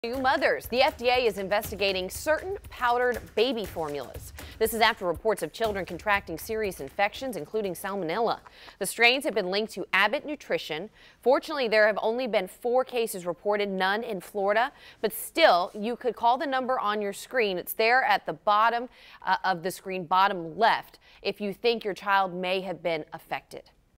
FDA: Do not use recalled infant formulas tied to infections
Parents are urged to stop using three popular infant formula brands with numbers and expiration dates because of reports of bacterial infection.